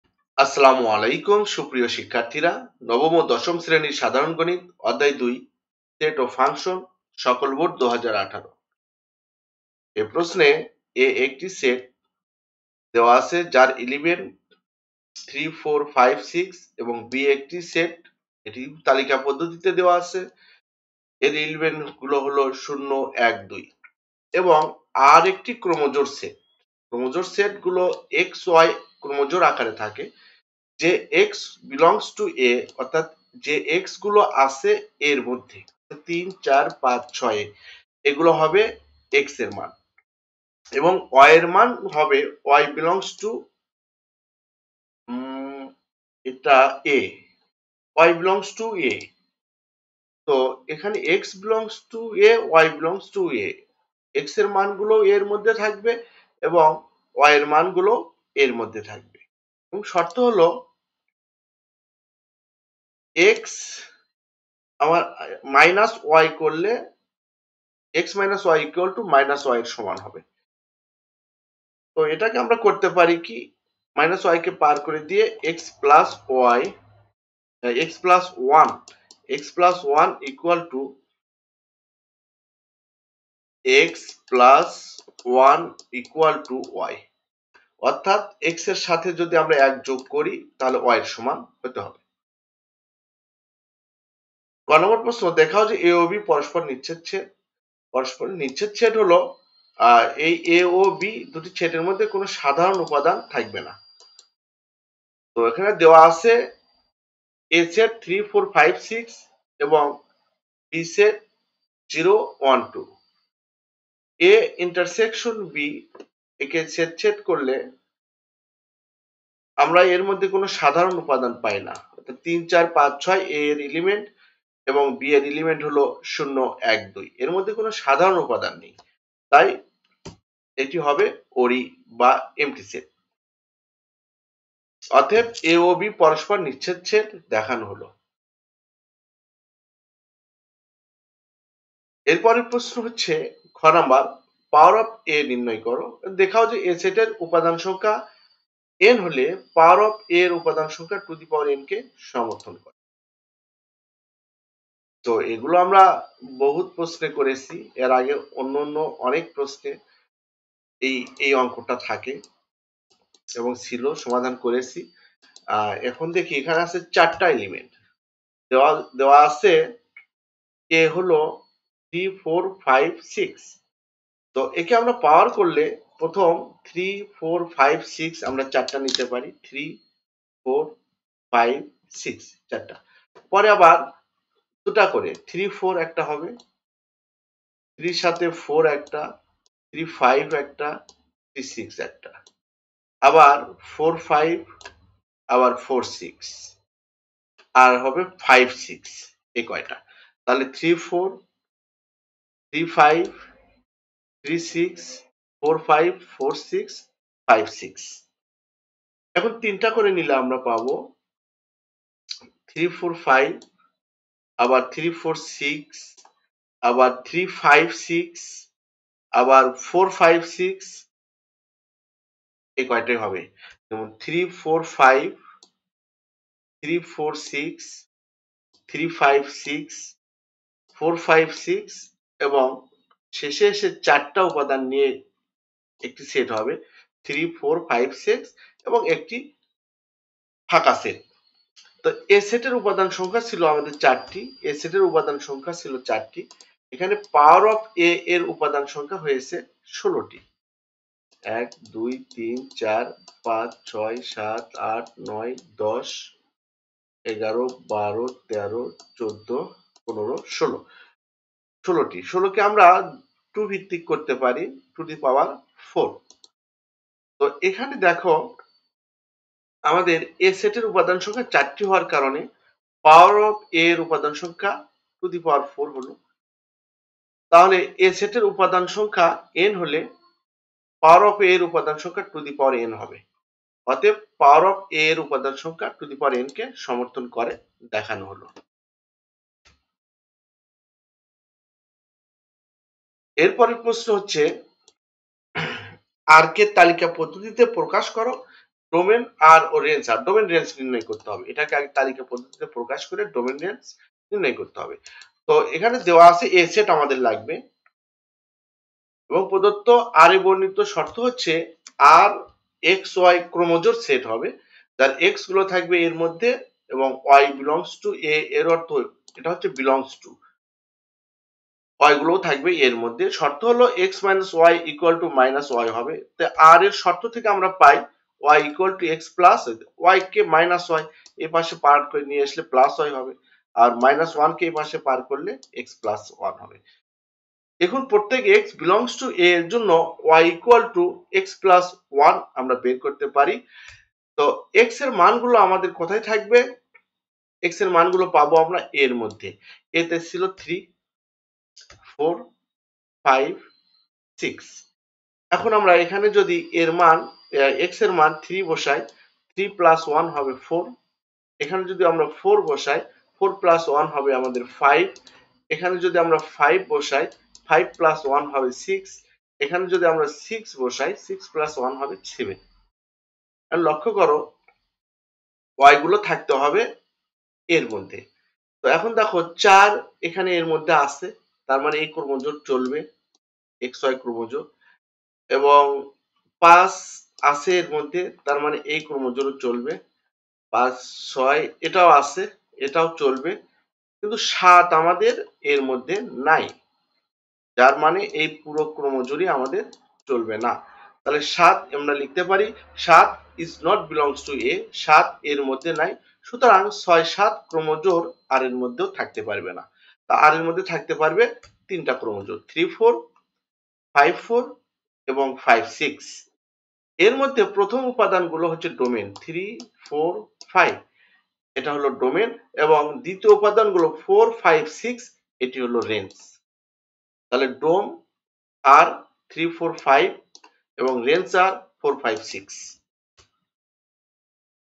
SSC Mathematics: Sets and Functions | All Boards 2018
Description:
Delve into the Sets and Functions chapter from the SSC syllabus applicable to all boards for the year 2018. This video offers detailed explanations and illustrative examples to help you grasp the key concepts in sets and functions, essential for mastering SSC mathematics.
more videos of this chapter-